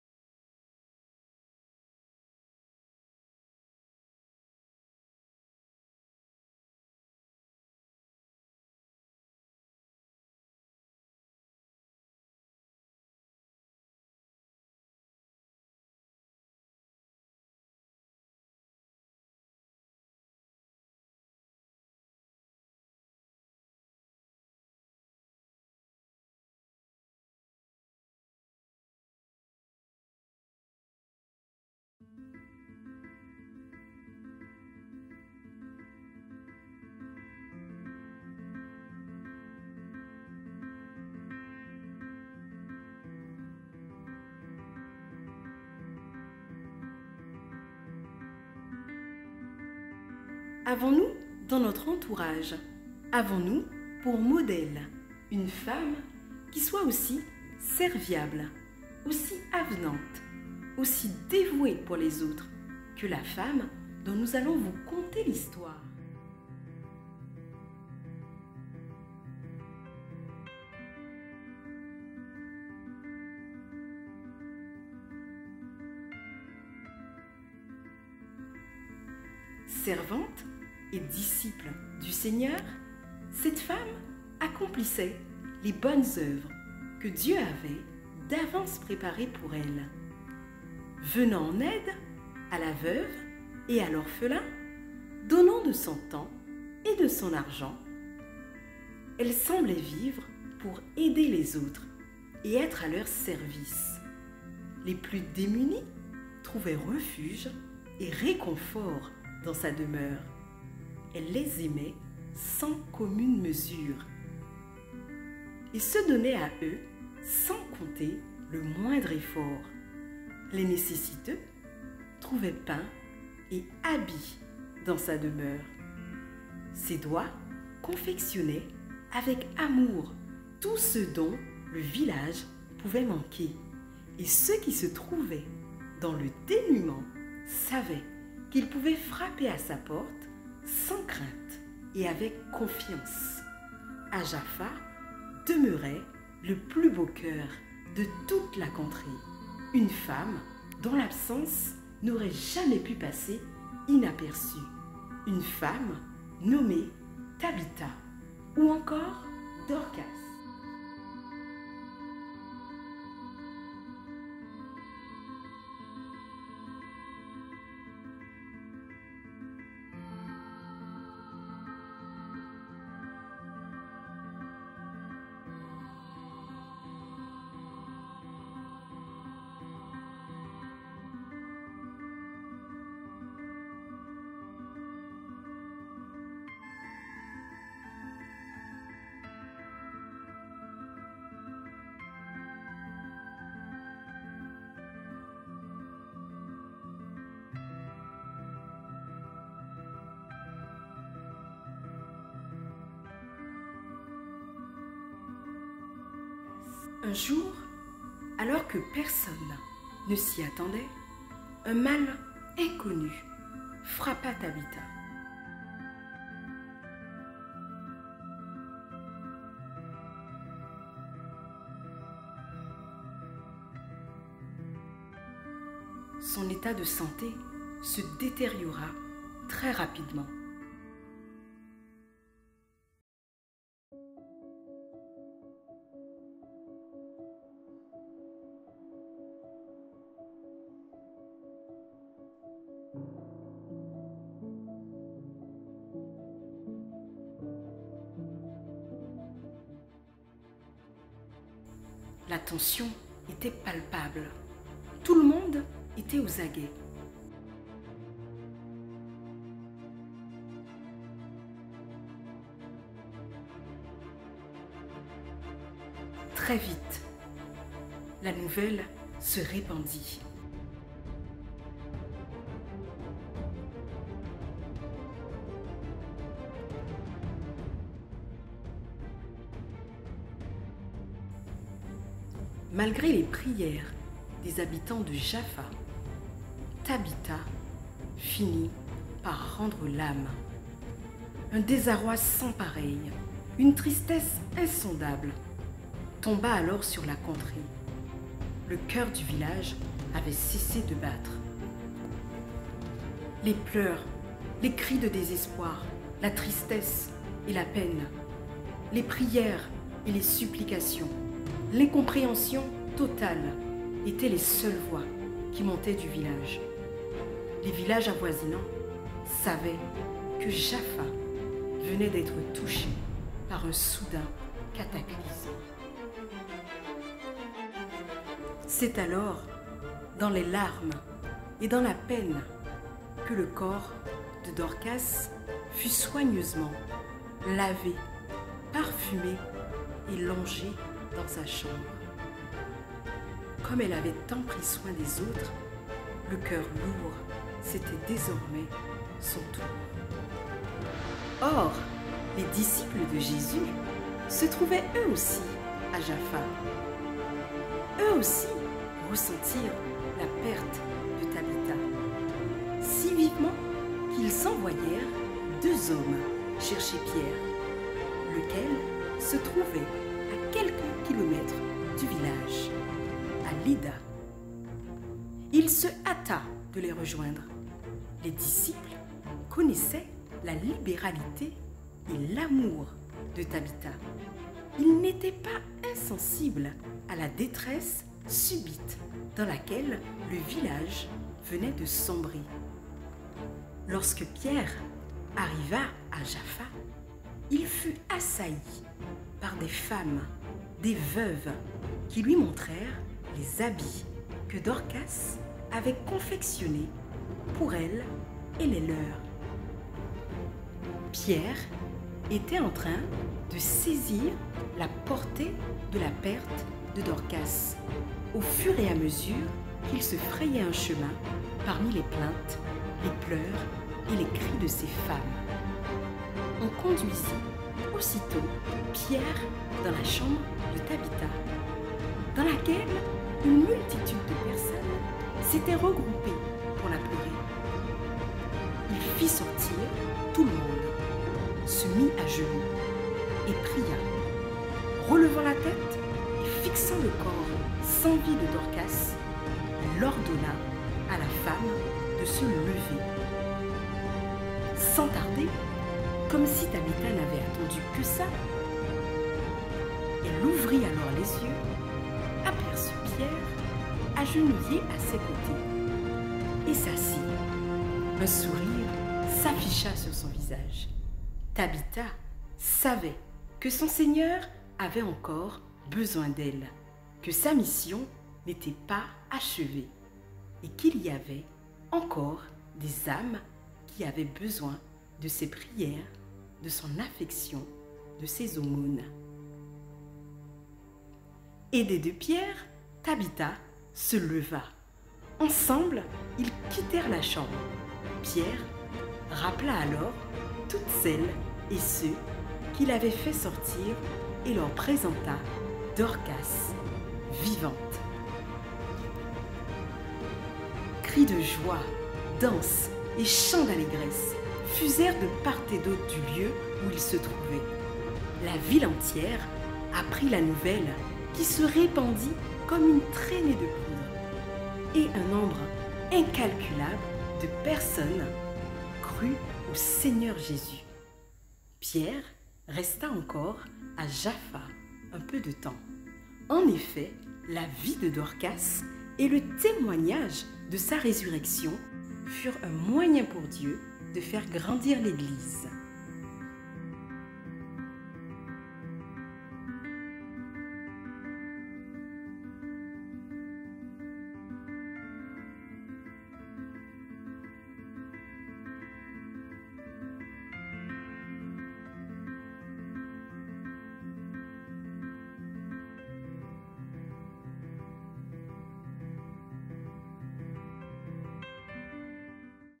L'histoire de DORCAS ✨ TABITHA une femme juste !
👉A l'occasion de notre culte des femmes qui a eu lieu le samedi 8 mai dont le thème était :

«Femme ta vie doit compter» 🔥

L'histoire s’appuie sur le livre Actes des apôtres chapitre 9 verset 36 à 43 de la Bible ,nous avons diffusé un court métrage mettant en scène l’histoire de Dorcas, une femme dont la vie a compté.🙇🏽‍♀️

Demeurez bénis !